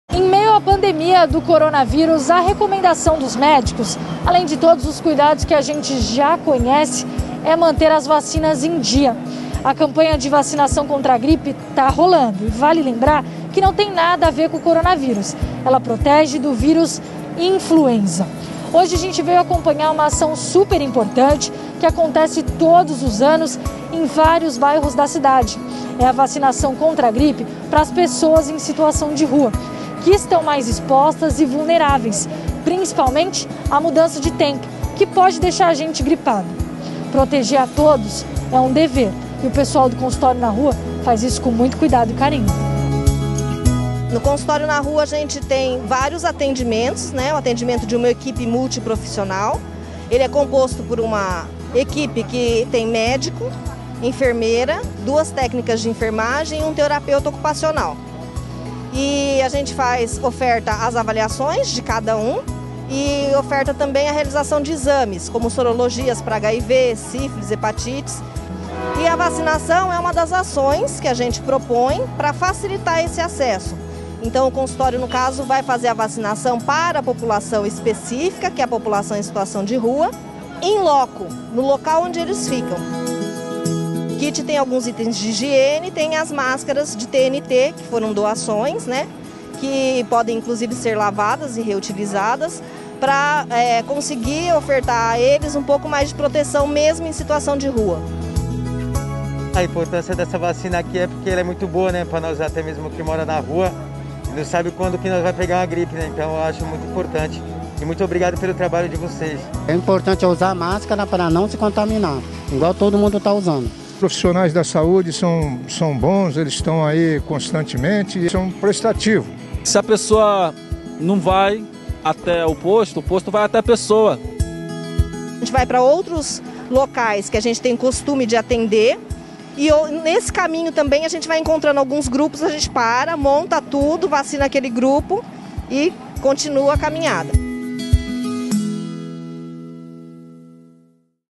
SANTOS VACINA POPULAÇÃO EM SITUAÇÃO DE VULNERABILIDADE CONTRA A GRIPE
Mais de 60 pessoas em situação de vulnerabilidade social foram vacinadas contra a gripe H1N1 por meio do programa Consultório de Rua, nesta sexta (15), nas proximidades do Mercado Municipal. No início da noite, o serviço teve continuidade pela região portuária, no Valongo. Além da imunização, houve distribuição de kits de higiene e de proteção ao novo coronavírus. Os produtos, incluindo máscaras faciais, álcool em gel, sabonetes e pastas de dente foram doados.